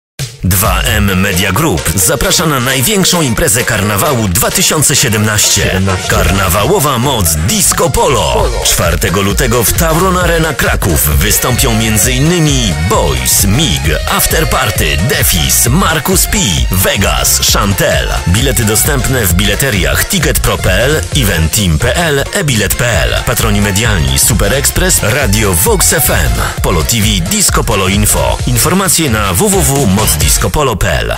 Karnawałowa Moc Disco Polo, TAURON Arena Kraków (04.02.2017)
Zapraszamy wszystkich na największą Karnawałowa Moc Disco Polo, która odbędzie się w Tauron Arena Kraków.
Partnerzy:
Polo tv, VOX FM, se.pl, Disco Polo, halodzwonek.pl, 4disco.tv, Amber Agency Fabryka Imprez